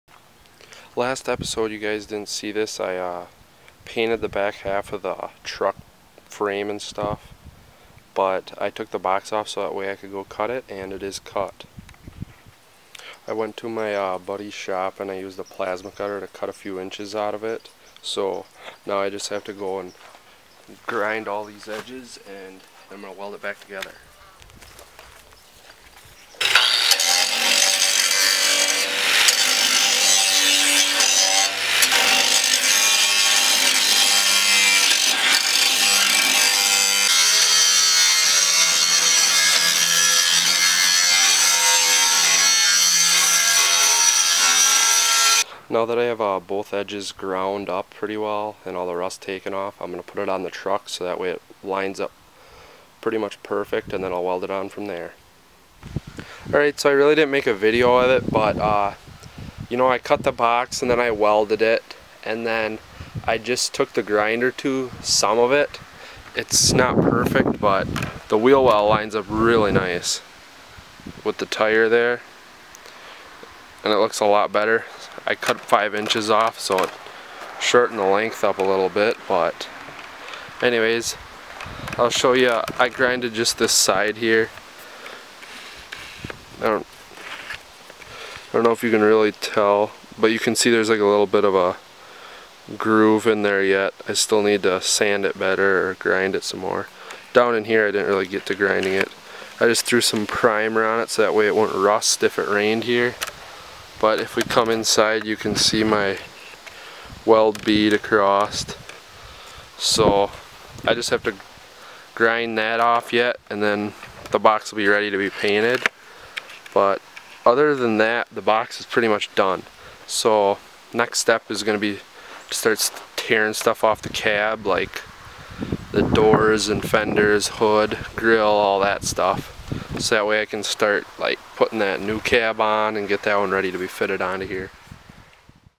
Offroad Truck Build: Chevy Scottsdale Ep. 2
Fitting the box up on the truck.  Doing some cutting, welding, and grinding.